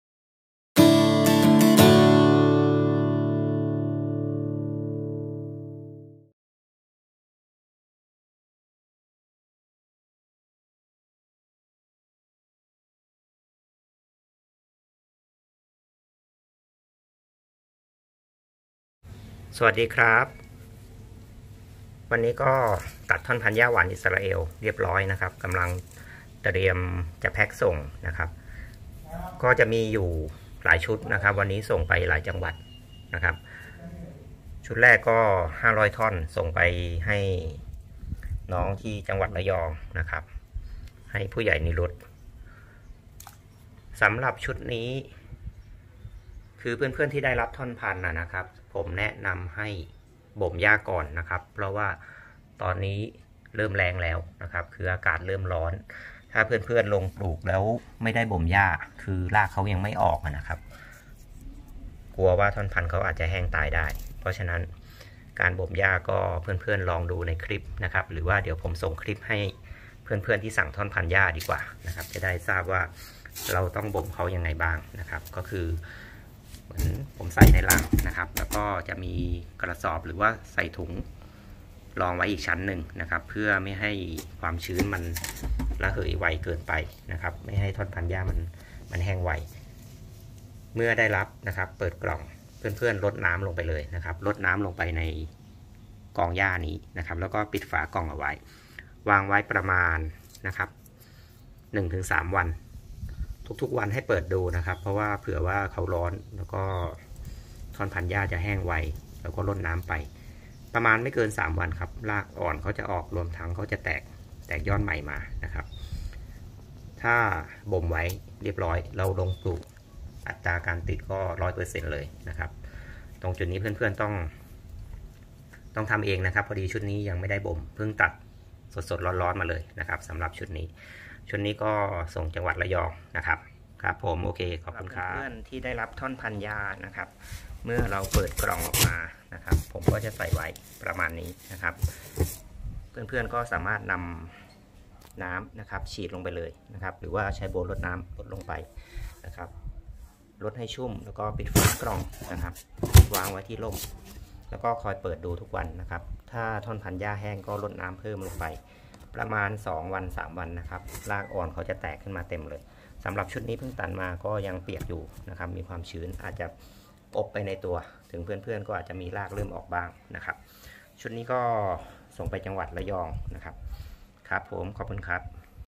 ท่อนพันธุ์หญ้าหวานอิสราเอล ขายส่งต.ชากโดน อ.แกลง จ.ระยอง
ตัดท่อนพันธุ์หญ้าหวานอิสราเอล เพื่อส่งไปปลายทาง ต.ชากโดน อ.แกลง จ.ระยอง หญ้าเลี้ยงสัตว์ สอบถามหญ้าหวานอิสราเอล ส่งข้อความและโทรฟรีด้วย LINE

สวนธนภัทรสกลนคร Line: Tanapat-Garden โทร.O9IO5I5839